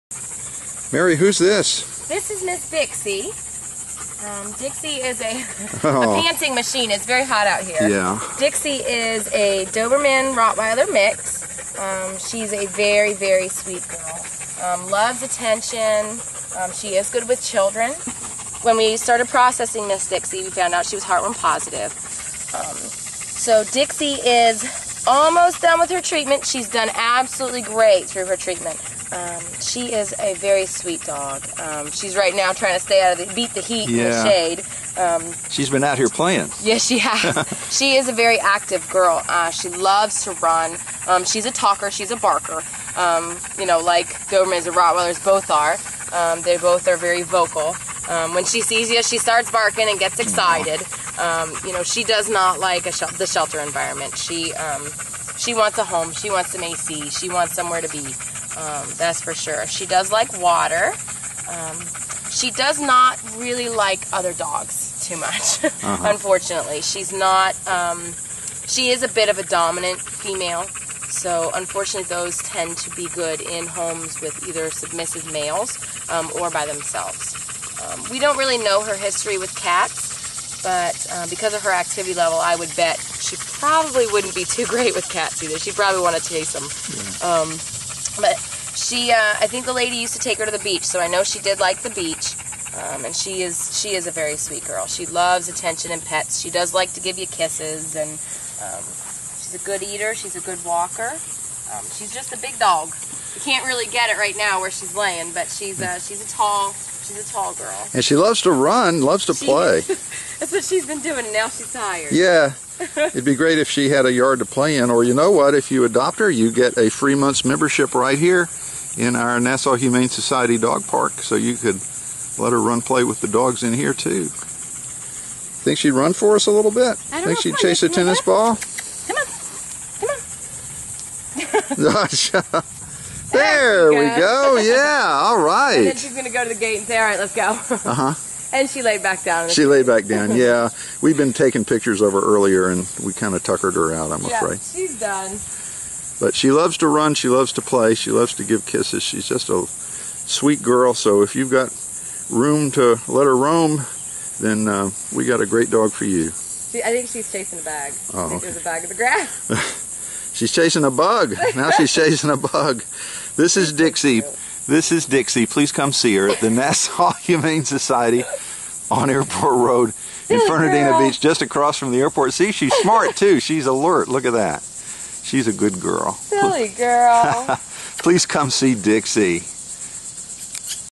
Meet Dixie!
Dixie is a sweet Doberman/Rottie mix who needs a great home! Please
come see her at the Nassau Humane Society, Fernandina Beach, FL (904)